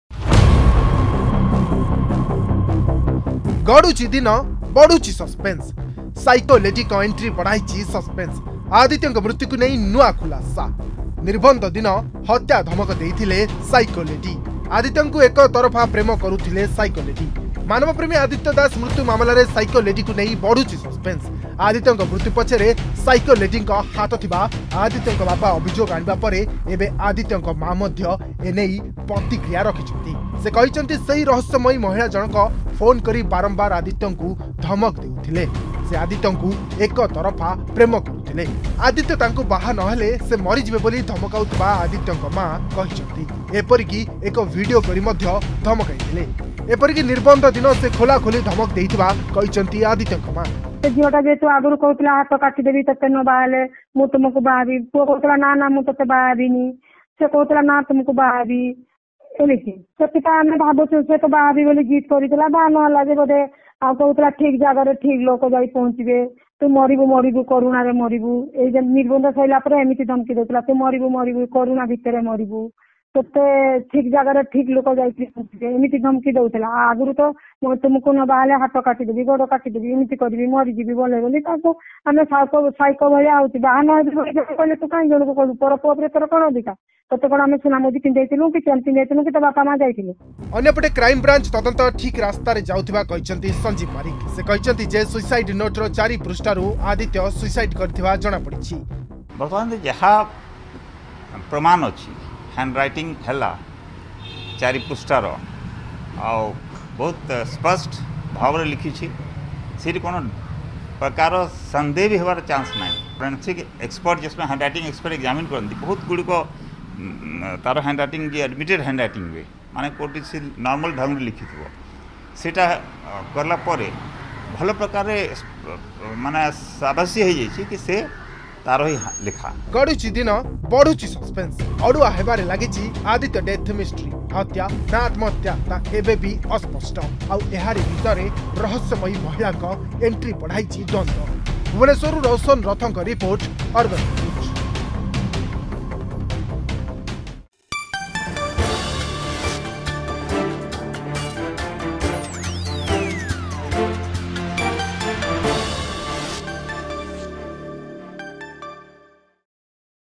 ଆଦିତ୍ୟଙ୍କ ମୃତ୍ୟୁକୁ ନେଇ ନୂଆ ଖୁଲାସା
ମାନବପ୍ରେମୀ ଆଦିତ୍ୟଦାସ ମୃତ୍ୟୁ ମାମଲାରେ  ସାଇକୋ ଲେଡିକୁ ନେଇ ବଢୁଛି ସସପେନ୍ସ । ଆଦିତ୍ୟଙ୍କ  ମୃତ୍ୟୁ ପଛରେ ସାଇକୋ ଲେଡିଙ୍କ ହାତ ଥିବା ଆଦିତ୍ୟଙ୍କ ବାପା ଅଭିଯୋଗ ଆଣିବା ପରେ ଏବେ ଆଦିତ୍ୟଙ୍କ ମାଆ ମଧ୍ୟ ଏନେଇ ପ୍ରତିକ୍ରିୟା ରଖିଛନ୍ତି